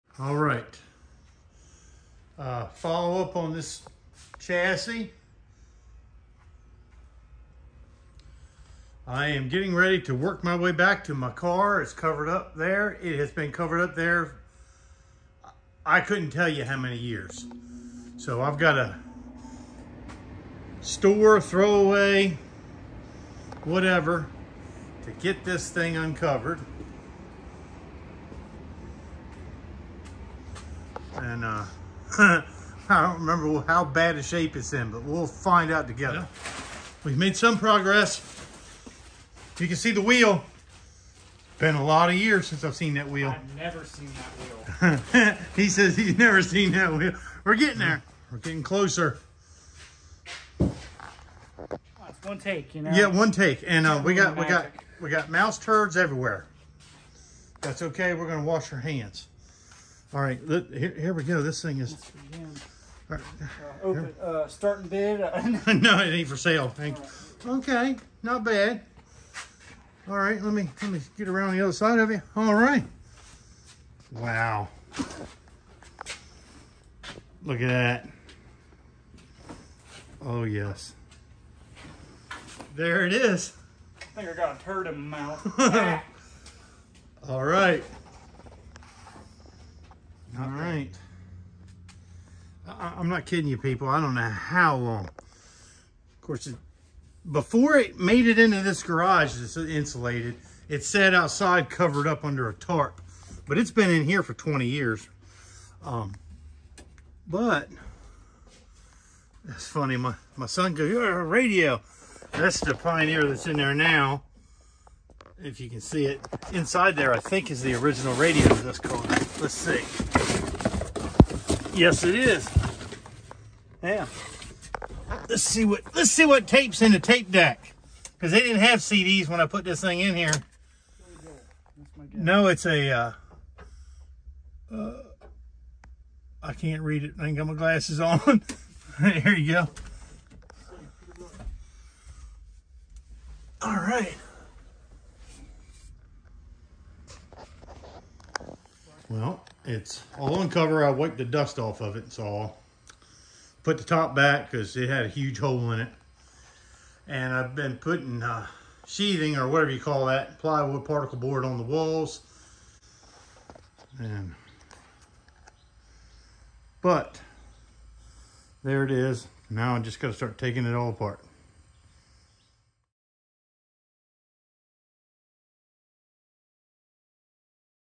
1965 Tempest Has been covered up for 20 Years!
My son and I uncovered my first car that has not been seen for probably 20. We are getting started on the rebuild that includes a new Scott's Hot Rod custom chassis.